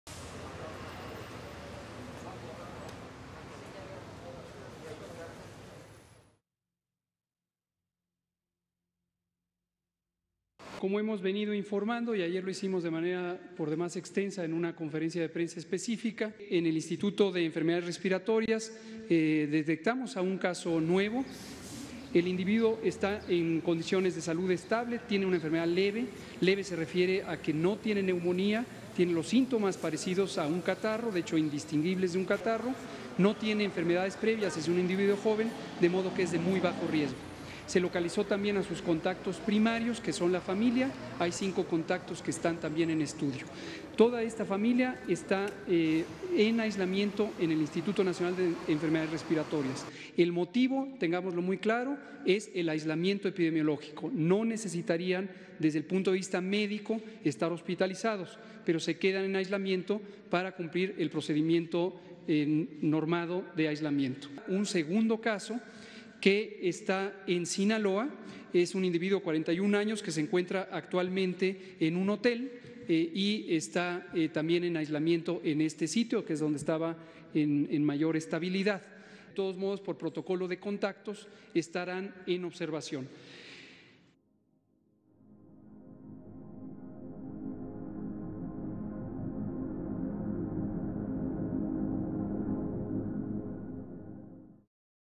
MÉXICO| CONFIRMA CASOS DE CORONAVIRUS
El
contagiado es un hombre de 35 años de Ciudad de México que viajó a
Italia y las autoridades están por ratificar un segundo caso en Sinaloa.